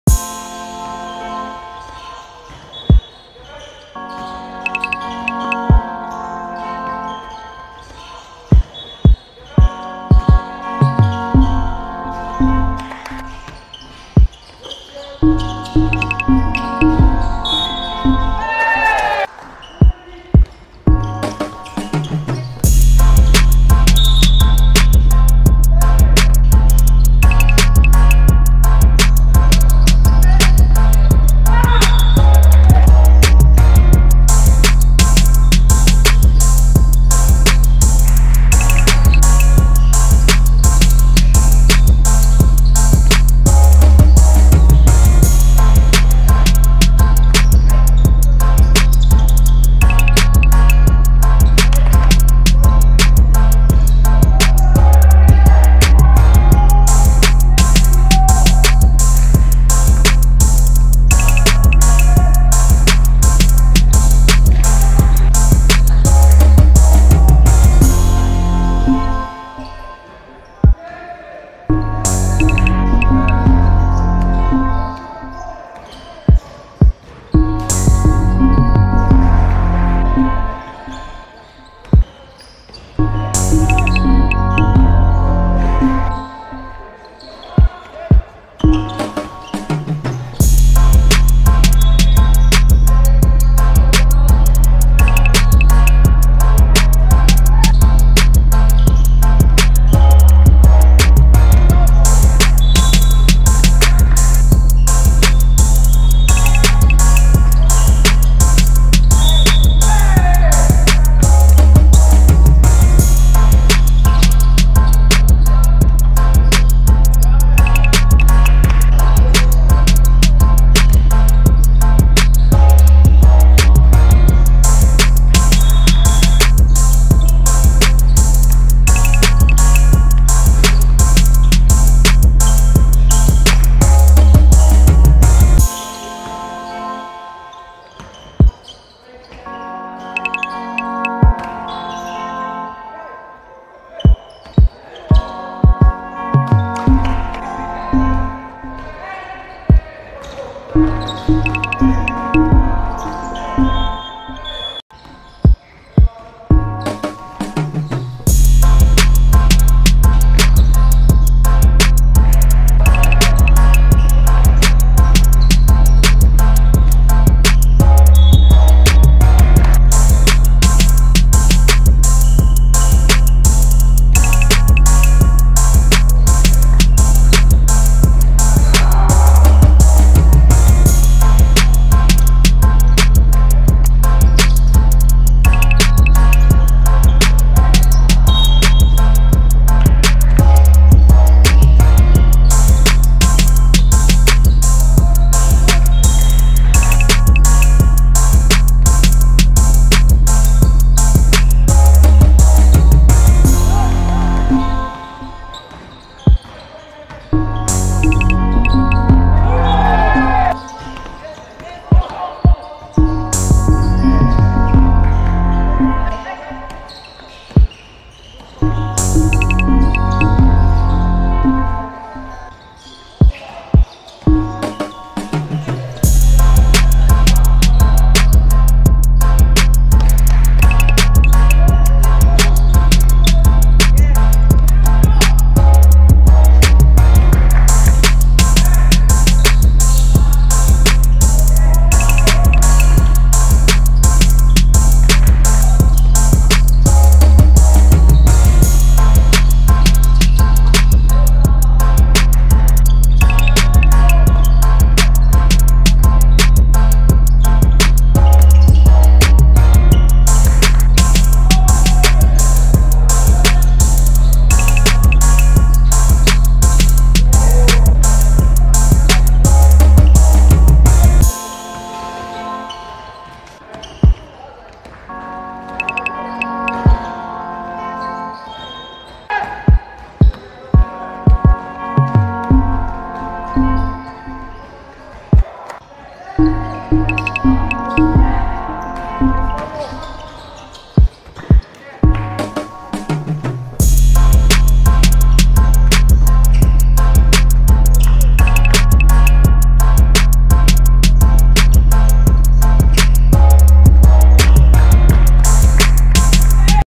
04 06 Kentucky vs Iowa – Division 1 Wiosna 2022
Kentucky 64:55 Iowa
H.Kędziorek 12pkt 10zb 11stl
W.Pełka 15pkt 11zb 3blk